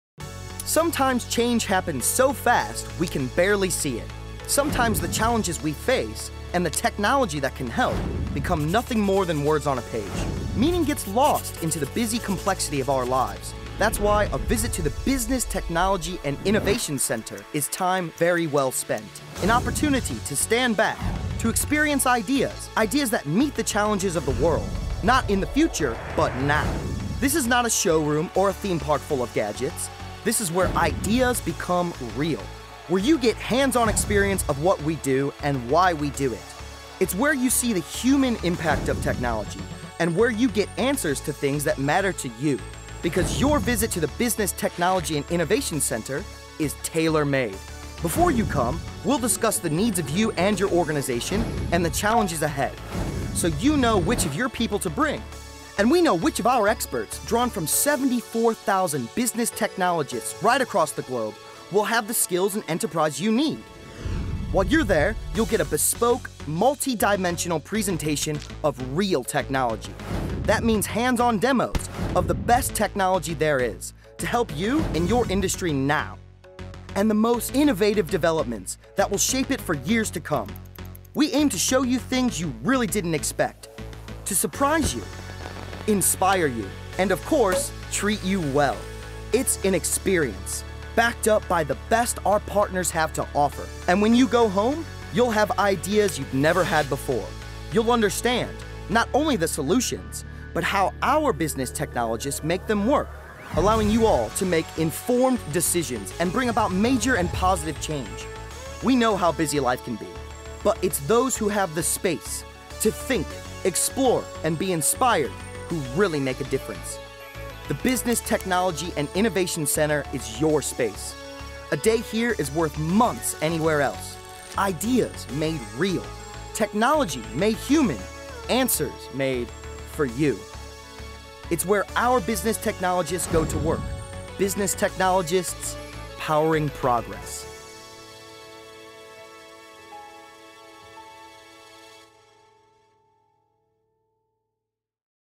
Atos Business Technology & Innovation Center
The Business Technology & Innovation Center (BTIC) was officially opened on September 26th, 2012 by Thierry Breton, Atos Chairman and CEO, and is located at Atos Headquarters in Bezons, close to Paris (France). An Atos Business Technology and Innovation Center is an exceptional business experience. It is a showcase for the future, offering you exclusive access to live examples of the best of Atos business solutions and innovation